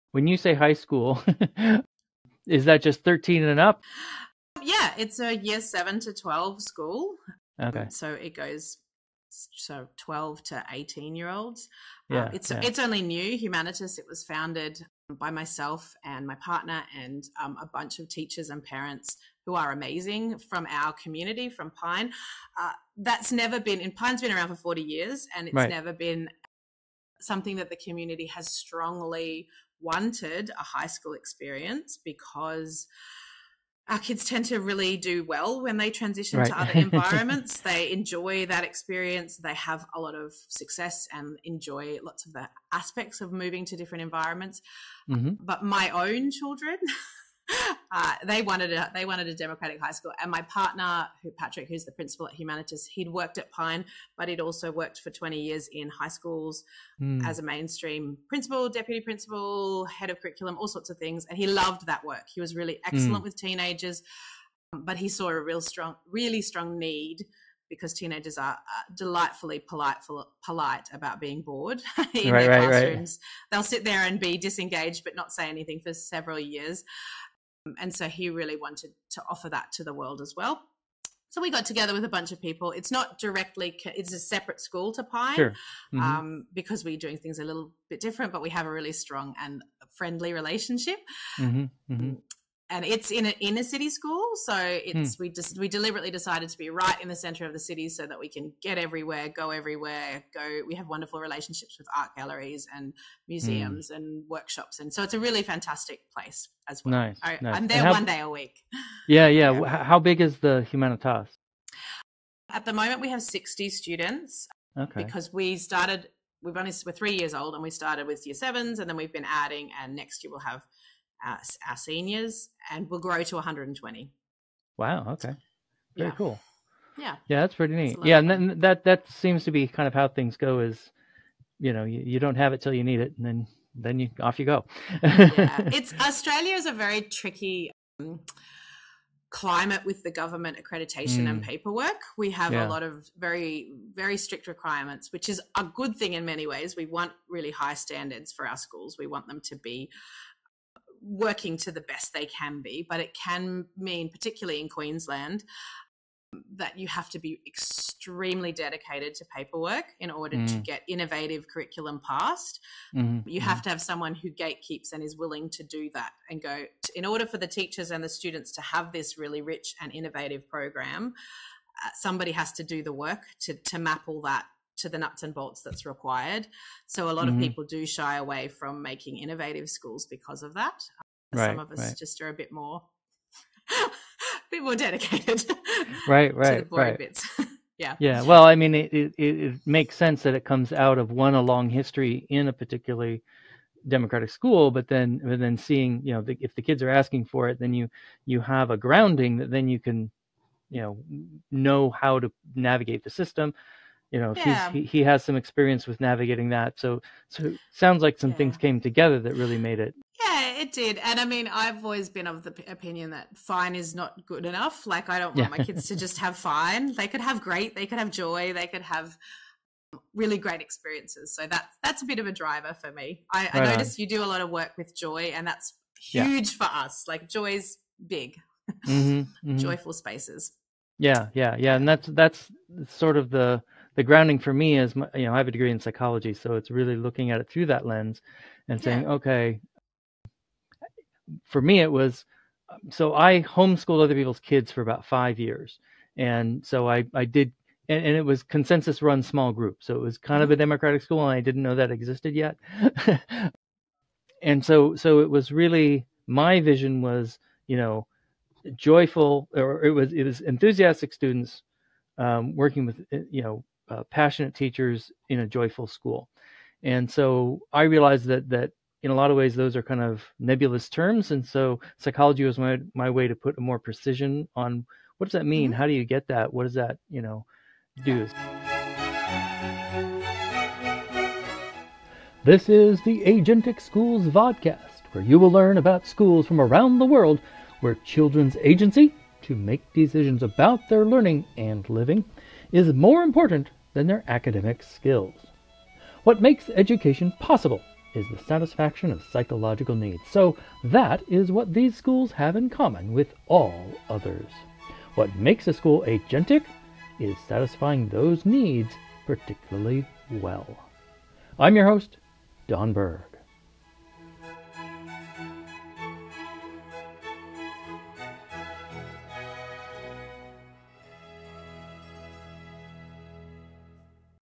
Cofounding Humanitas High- Mathilda Element - Pine Community School & Humanitas High School-S2E12x3
Guest: Mathlida Element - Pine Community School & Humanitas High School, Arana Hills, Brisbane, Queensland, Australia